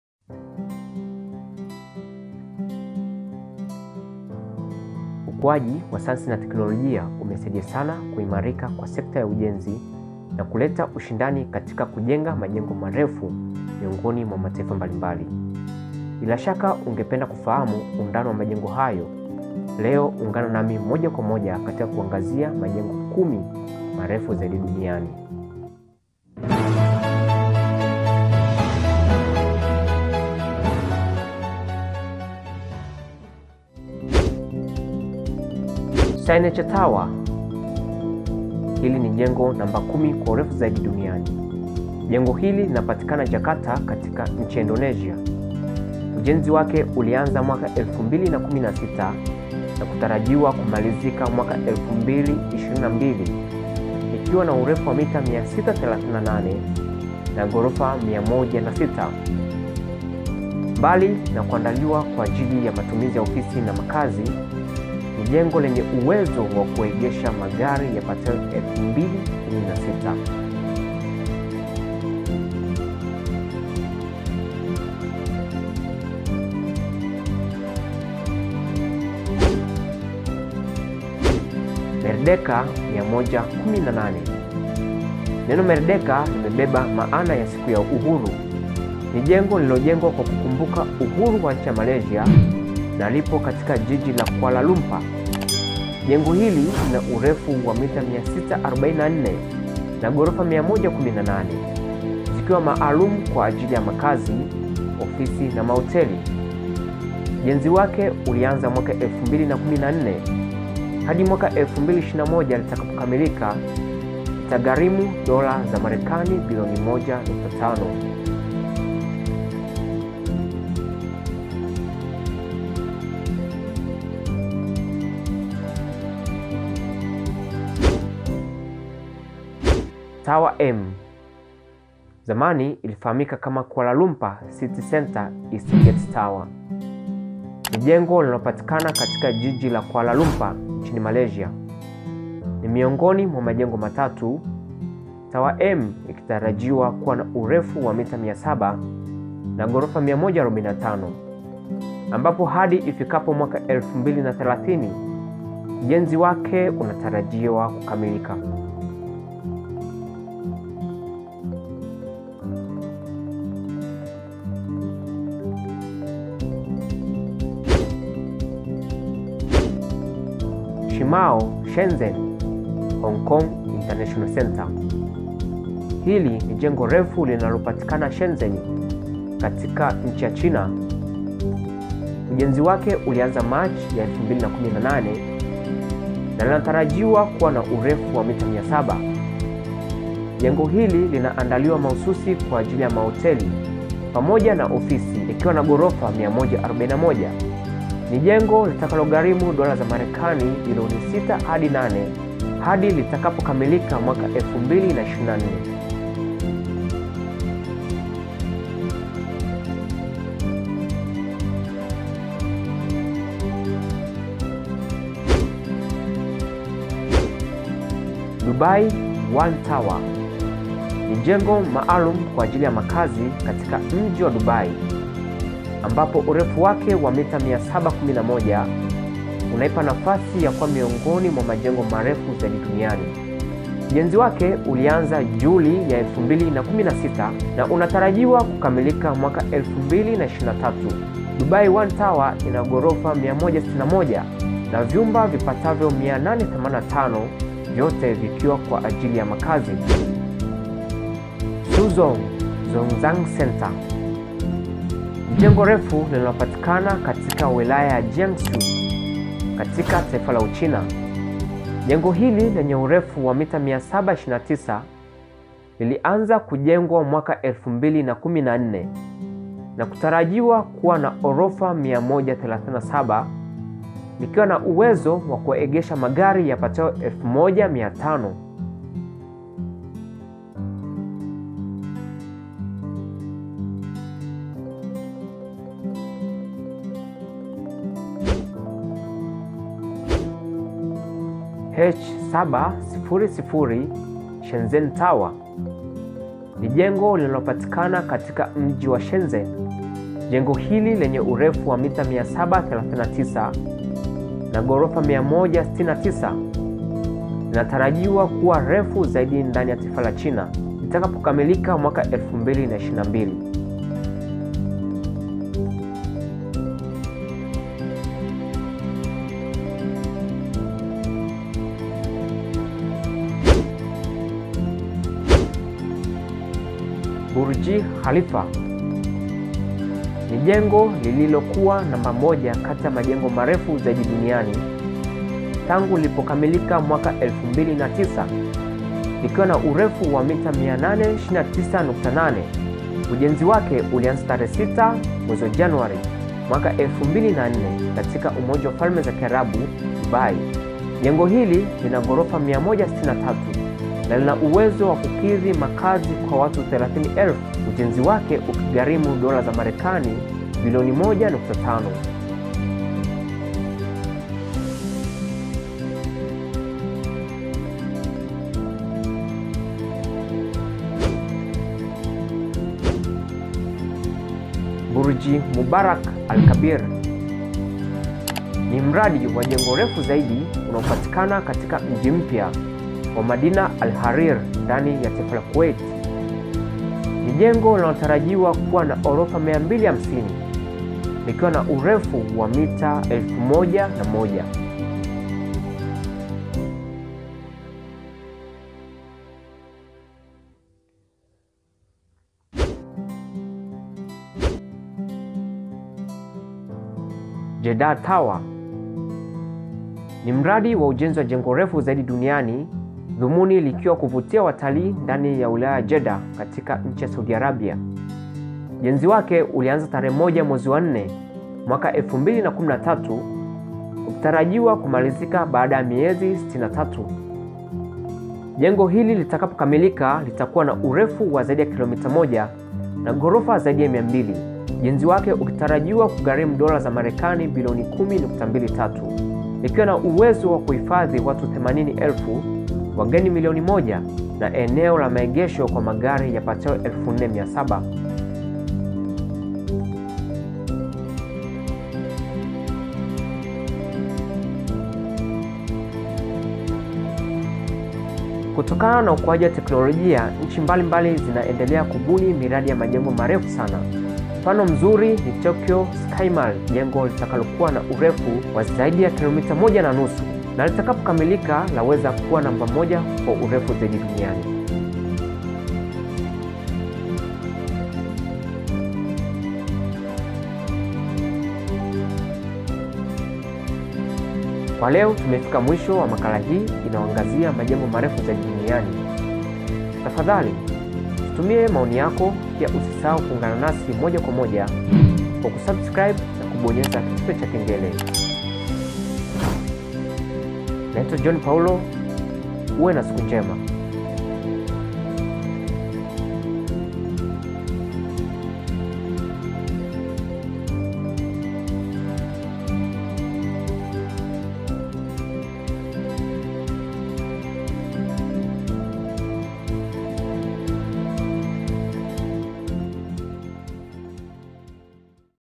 HAYA NDIO MAJENGO 10 MAREFU ZAIDI DUNIANI KWA MWAKA 2020-2021!
Hii ni orodha ya majengo kumi yanayoongoza kwa urefu duniani. Wapi yalipo na undani wake utaupata katika makala hii.

 # John Paulo  Stories I Updates #

# John Paulo   Stories I Update #